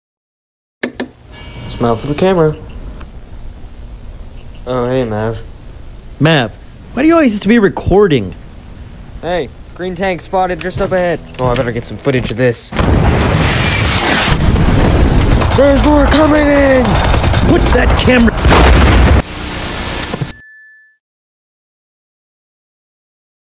Army men Found Footage
I’m trying a new series where I have army men documenting their experiences on the frontlines. Hope you enjoy!